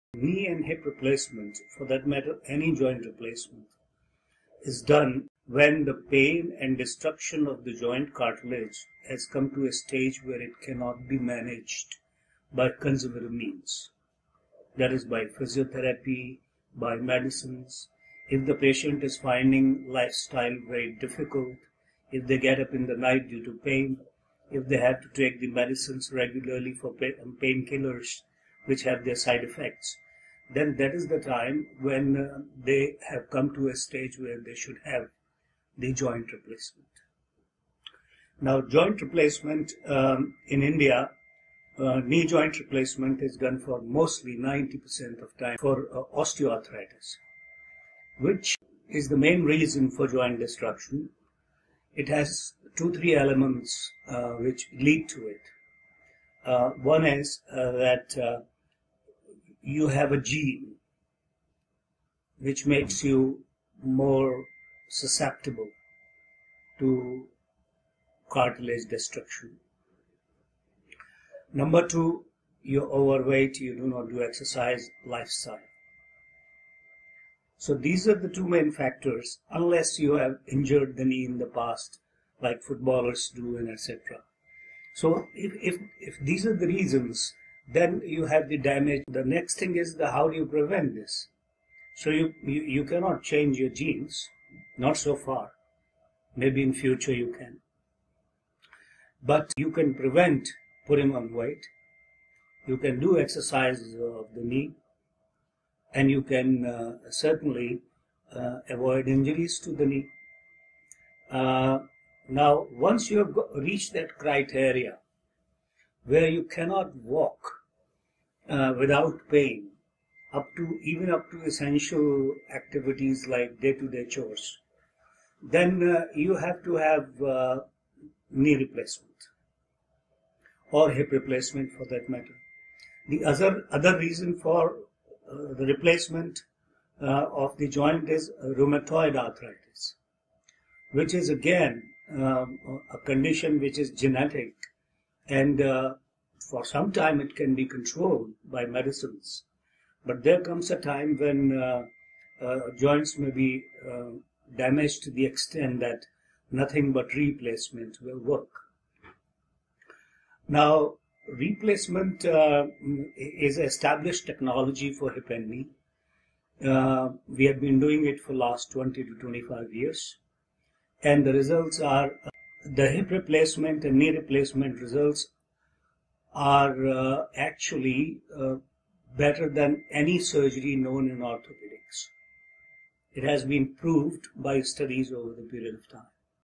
Dr. K. B. Attri - Joint Replacements
Dr. K. B. Attri, Senior Consultant, Orthopaedic Surgery at Indraprastha Apollo Hospital enlightens us about joint replacement.